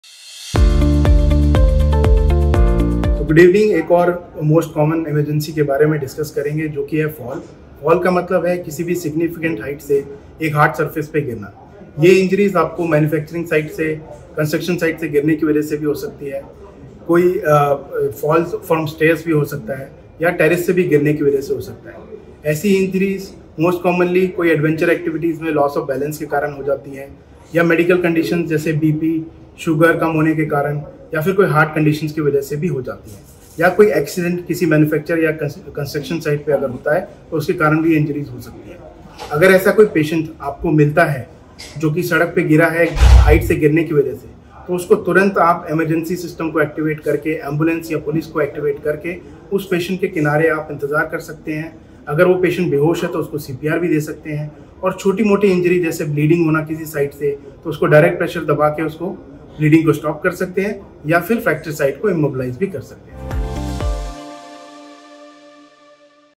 What to do during Fall Emergency? | Dr Kapil Gupta | Manipal Hospital Delhi
Falling incidents are quite common, and pose a great danger to the chances of survival for any patient. These situations can happen anywhere, from your place of work to your home, and even while traveling. Often it is also seen that an underlying condition also plays a pivotal role in causing these incidents.

On the occasion of WorldEmergencyMedicineDay, Dr Kapil Gupta, Consultant - Emergency Medicine at Manipal Hospital Dwarka explains in this video how you can help a fall victim in a time sensitive situation where you can save someone’s life.